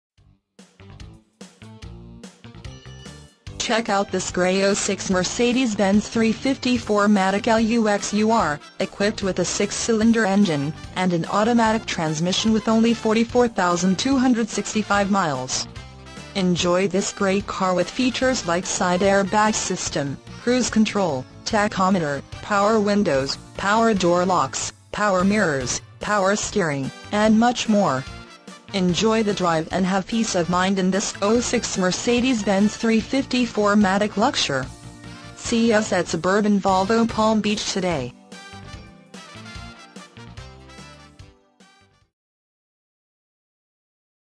2006 Mercedes-Benz C-Class C35 Suburban Volvo Palm Beach
2006 Mercedes-Benz C-Class C35

 44,265 Miles

VIN - WDBRF87H36F748116

Check out this Gray 2006 Mercedes-Benz C-Class C350 4MATIC Luxur, equipped with a 6 Cyl. engine  and an automatic transmission with  only 44,265 miles. Enjoy this great car with features like Side Air Bag System, Cruise Control, Tachometer, Power Windows, Power Door Locks, Power Mirrors, Power Steering, and much more. Enjoy the drive and have peace of mind in this 2006 Mercedes-Benz...